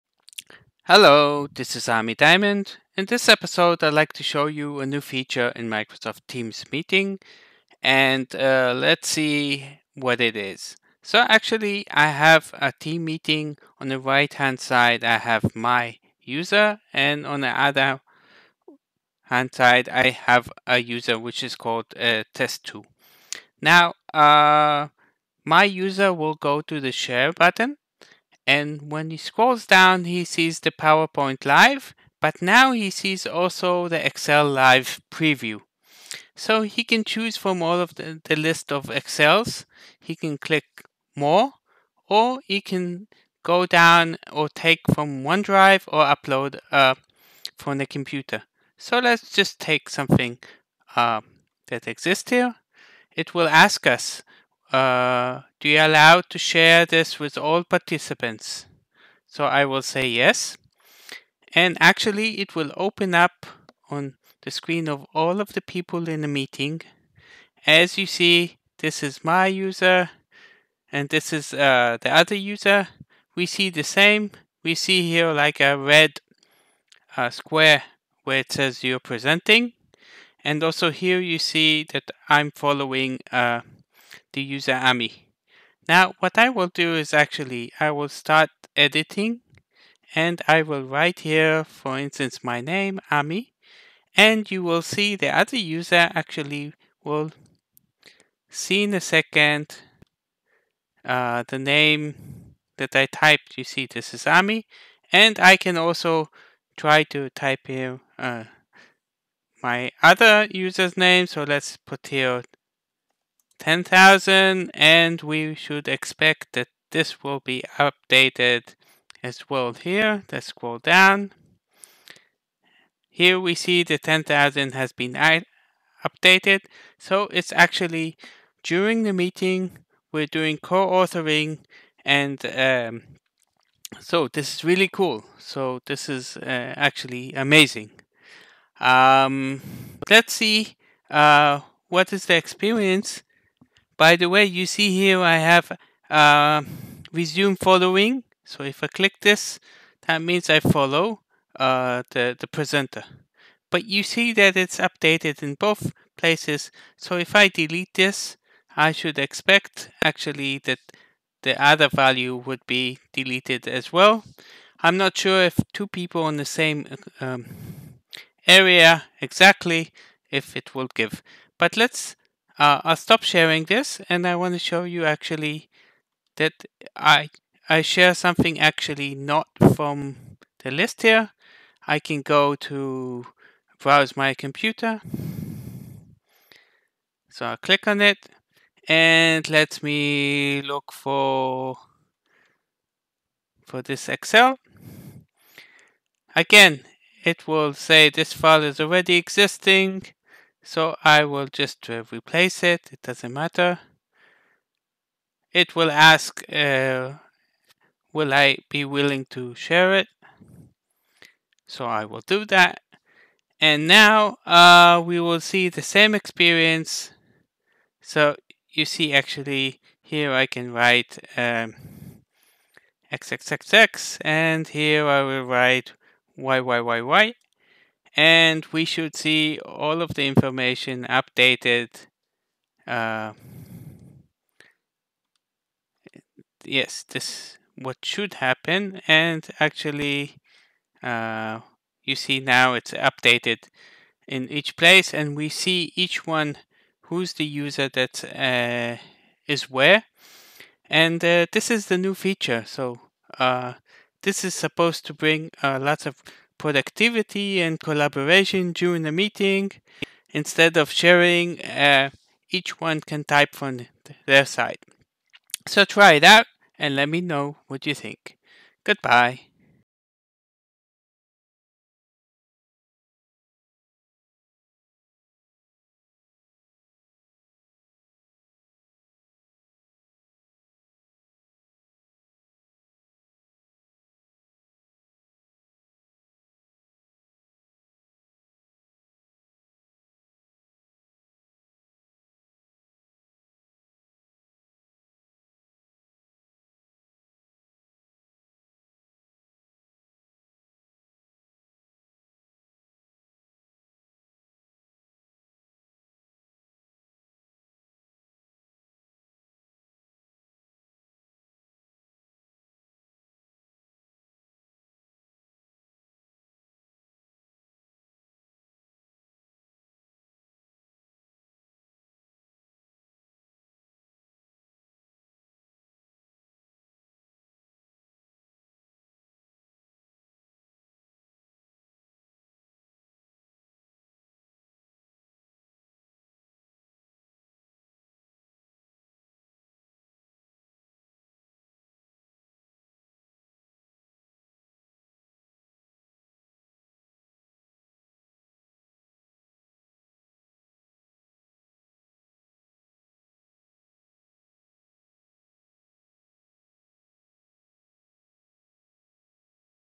How to work with Excel live in Microsoft teams meetings ?
Excel Live is a new feature that is currently in Public preview and it enables teams meeting members to work together during the meeting on one single file simulations
In this video I will show you look and feel of the feature